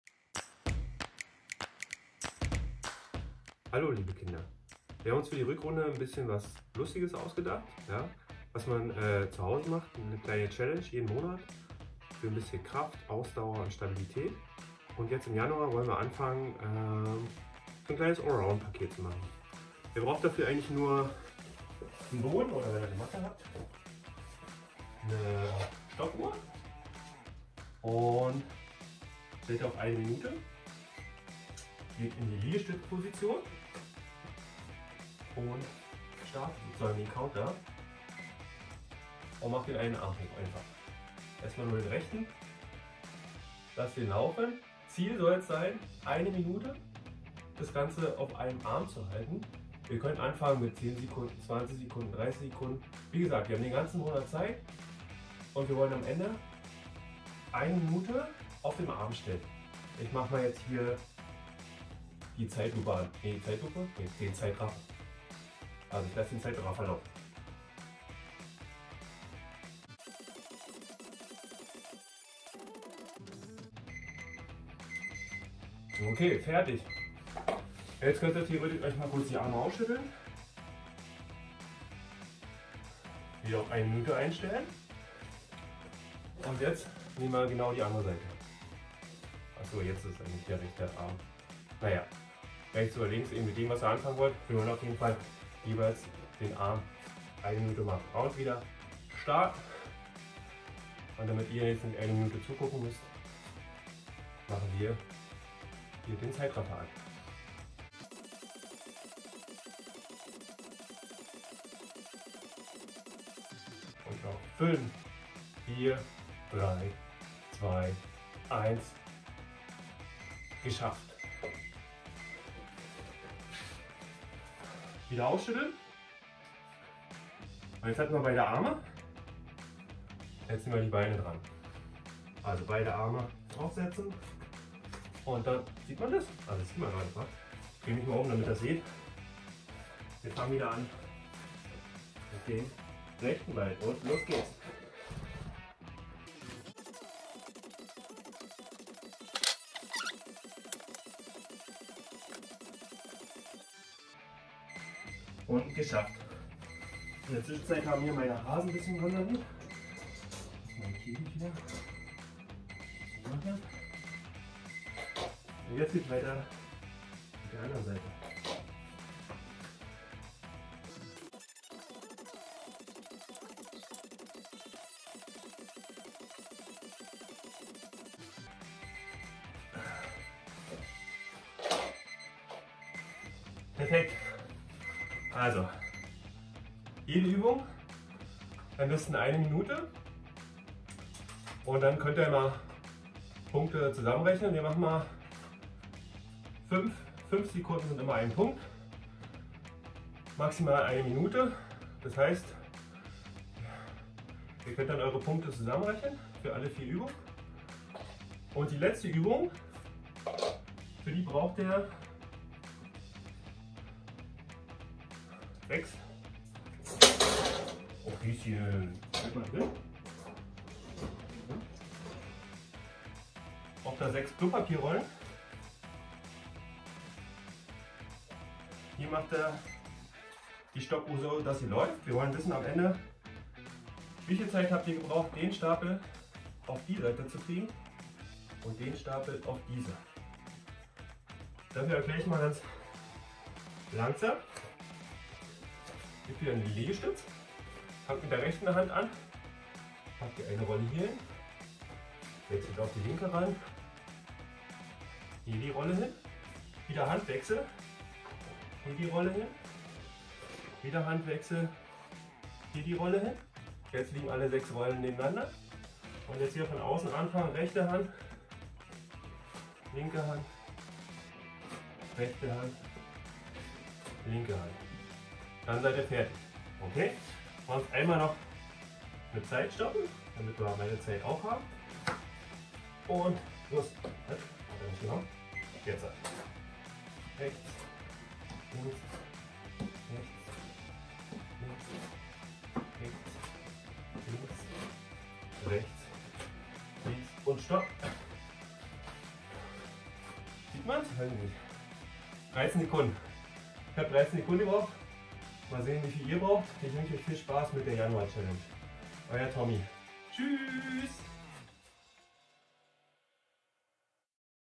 G-Jugend-Challenge: Januar (08.01.2023)
Wir haben uns für die Rückrunde etwas für unsere G-Jugend überlegt. Es wird jeden Monat eine paar Übungen für zu Hause geben. Die Übungen sollen vor allem Spaß machen und heimlich etwas für die Körperspannung, die Motorik und die Koordination tun. Der Weg ist dabei das Ziel. 😉 Am besten jeden Tag oder jeden zweiten Tag immer wieder versuchen bis jeder/jede am Monatsende als Meister:in dasteht.

Kurze Erklärung zur Januar-Challenge:
5 Sekunden - 1 Punkt
10 Sekunden - 2 Punkte
60 Sekunden - 12 Punkte

Macht bei vier Übungen eine Maximalpunktzahl von 48 Punkten. Und die Zeit für die fünfte Übung.

Die „größeren“ Kids und/oder Eltern sind natürlich eingeladen mitzumachen. ❤️💙⚽️

Über eure Ergebnisse würden wir uns freuen. Als Zahlen, Bild oder als Video 😉